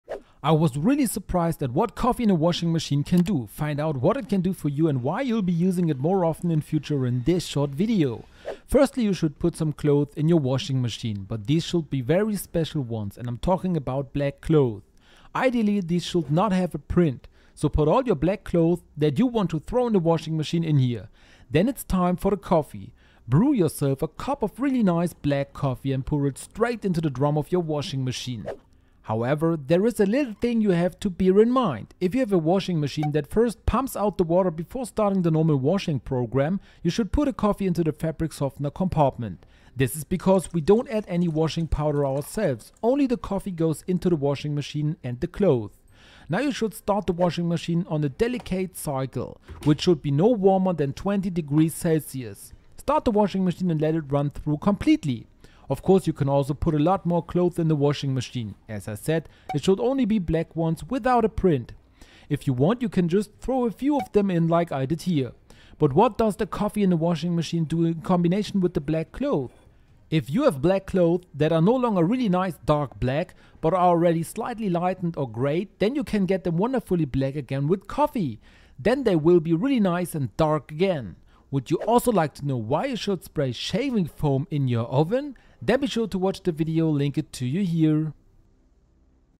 Dump Coffee in your Washing Machine and WATCH WHAT HAPPENS💥(Amazing)🤯
Have you ever poured coffee into your washing machine? Here I'll show you why you absolutely should!

You've probably never thought of pouring coffee into your washing machine. In this video, I show you why you should definitely do exactly that.

To do this, you need to make a cup of black coffee and then pour it into your washing machine. You should now add dark clothes and only dark clothes. Even without a colored print.

Then turn on the washing machine at a low temperature. This will actually make the dark clothes that have grayed out over time really dark again.